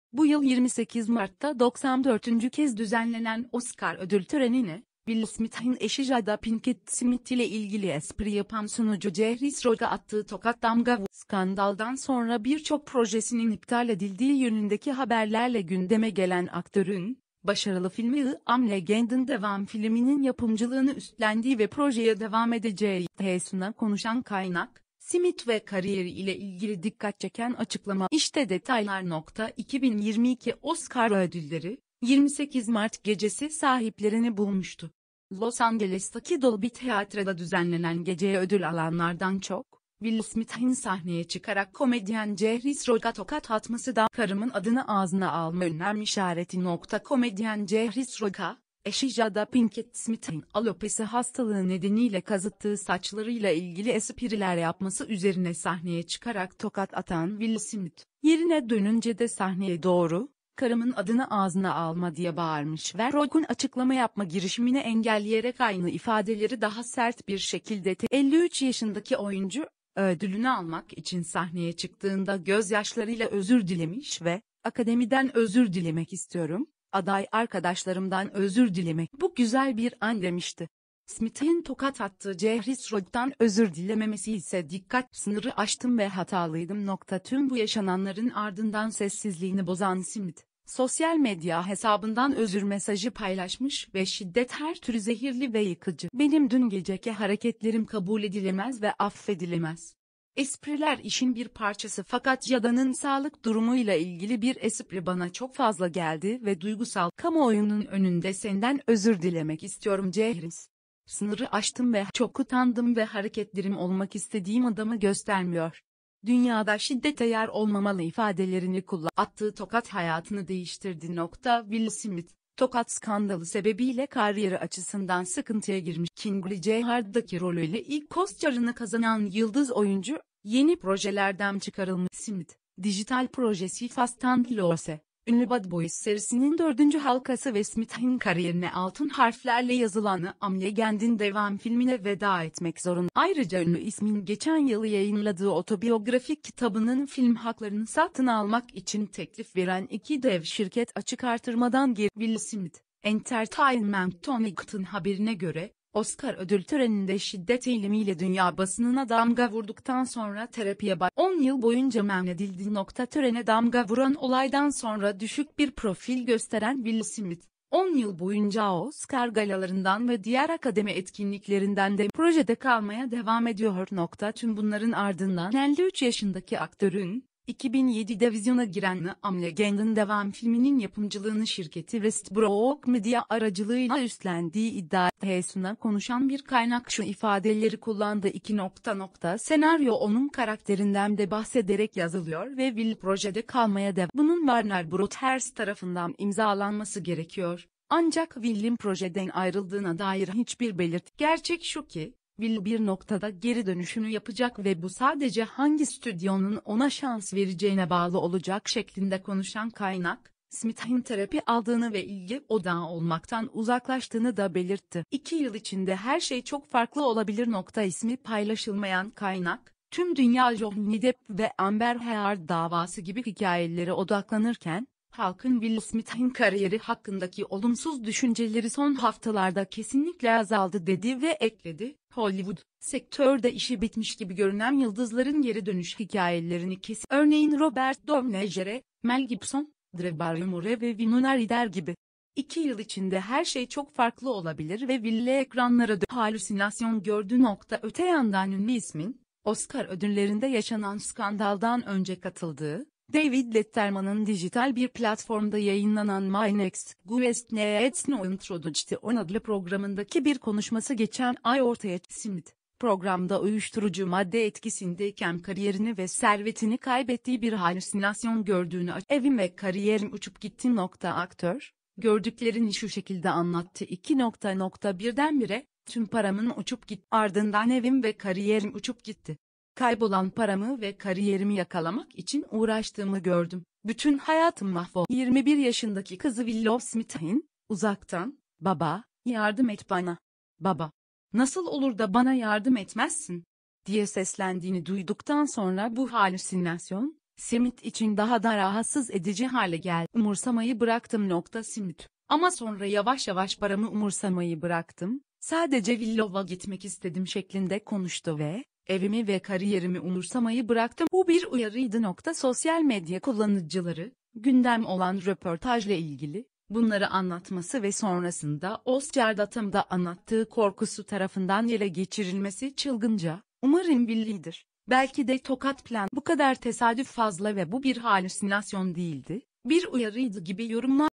📷Will Smith 'tokat skandalı' sonrası ekranlara dönecek mi? Dikkat çeken açıklama
Bu yıl 28 Mart'ta 94'üncü kez düzenlenen Oscar ödül törenine; Will Smith'in eşi Jada Pinkett Smith ile ilgili espri yapan sunucu Chris Rock'a attığı tokat damga vurmuştu. Skandaldan sonra birçok projesinin iptal edildiği yönündeki haberlerle gündeme gelen aktörün, başarılı filmi 'I Am Legend'ın devam filminin yapımcılığını üstlendiği ve projeye devam edeceği iddia edildi. The Sun'a konuşan kaynak, Smith ve kariyeri ile ilgili dikkat çeken açıklamalarda bulundu. İşte detaylar...